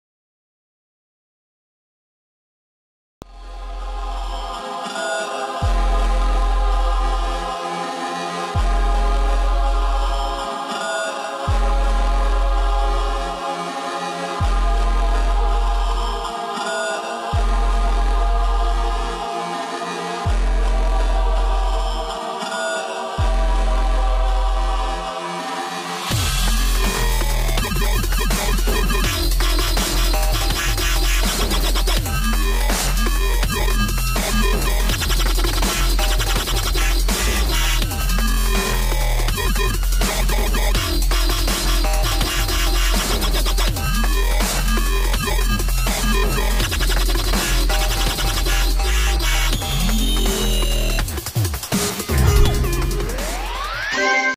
Celsius Circles update: new backround effect
Credit to abdo Kh for his/her suggestion for the backround effect! I credited his/her suggestion on 0:32 thank you for the suggestion by the way